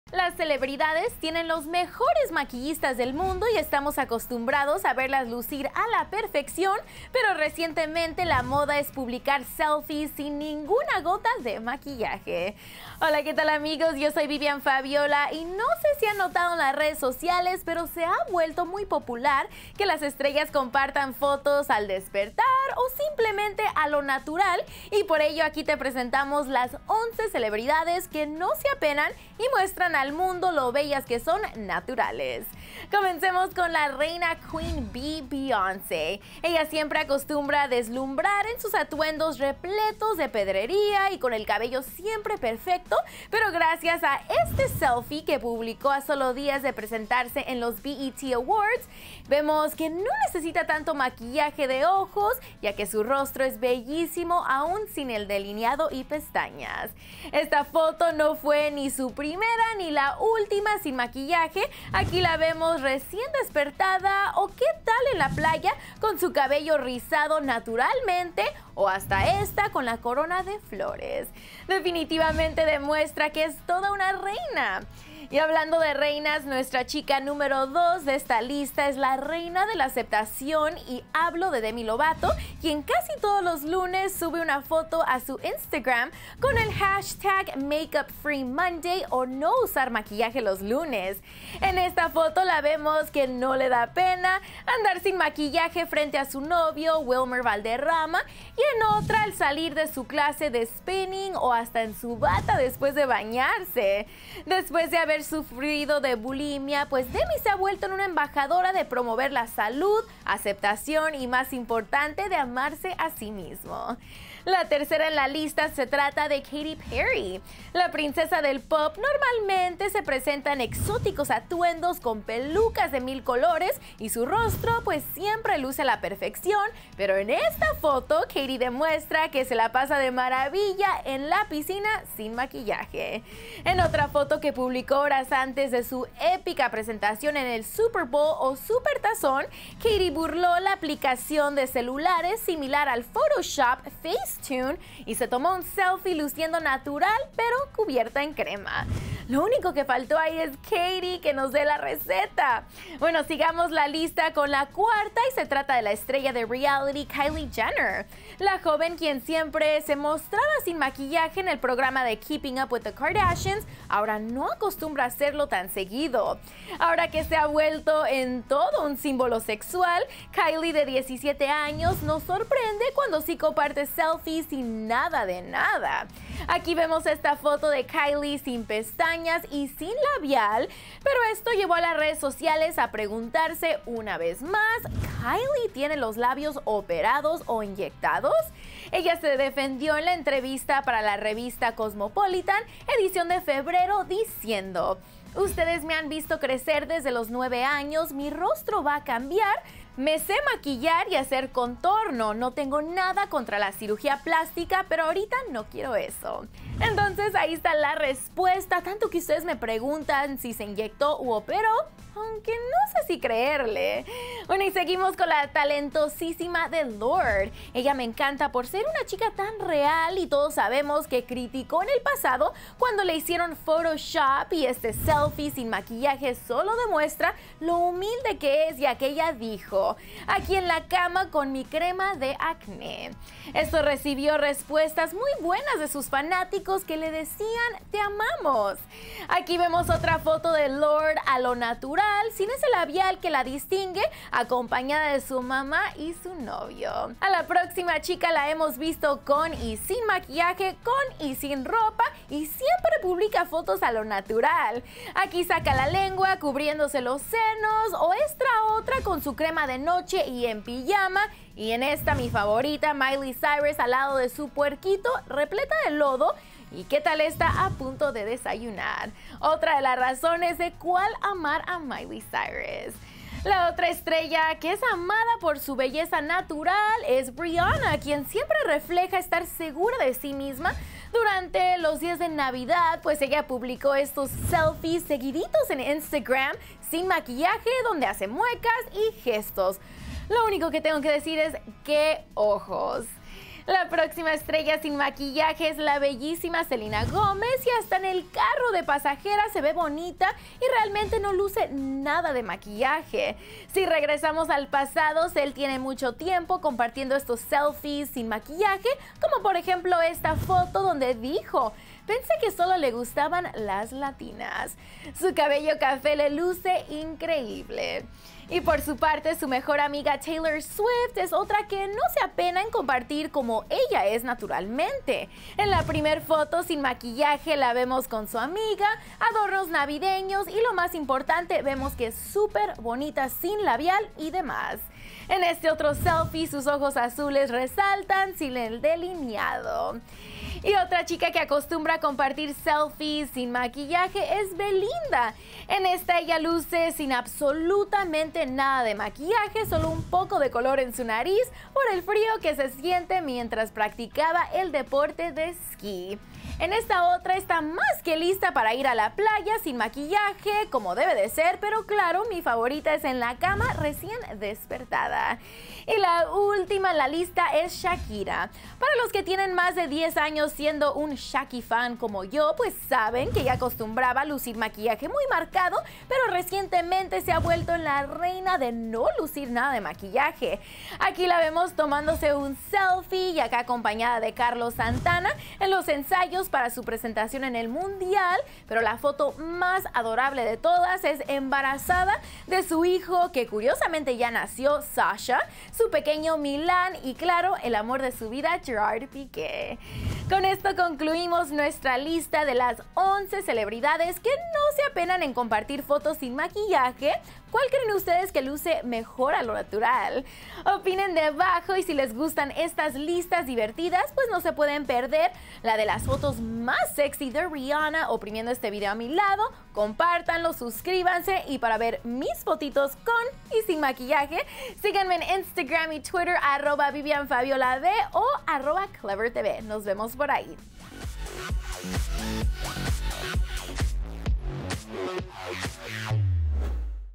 11 Celebridades Sin Una Gota de Maquillaje
11 Celebridades Sin Una Gota de Maquillaje
Las celebridades tienen los mejores maquillistas del mundo y estamos acostumbrados a verlas lucir a la perfección pero recientemente la moda es publicar selfies sin ninguna gota de maquillaje.
Se ha vuelto popular que las estrellas compartan fotos al despertar o simplemente a lo natural y por ello aquí te presentamos las 11 celebridades que no se apenan y muestran al mundo lo bellas que son, naturales.